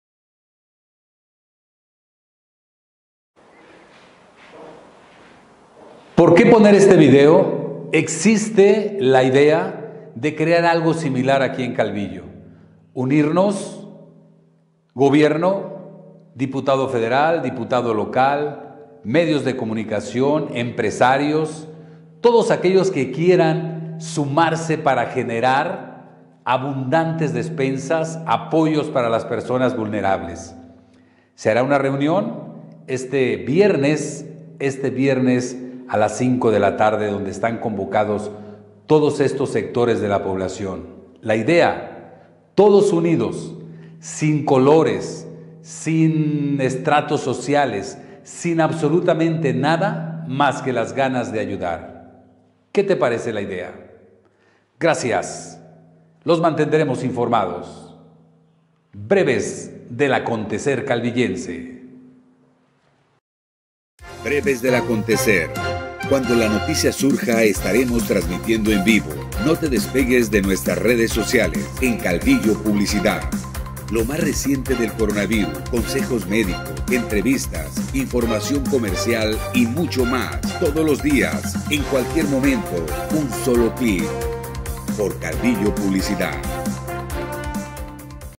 TODOS POR CALVILLO
Todos por Calvillo, es un interesante proyecto idea del Párroco Francisco García Zavala, quien propone unir fuerzas entre Gobierno, Iglesia, empresarios, medios de comunicación y quien quiera sumarse, en apoyo a personas vulnerables en esta contingencia médica.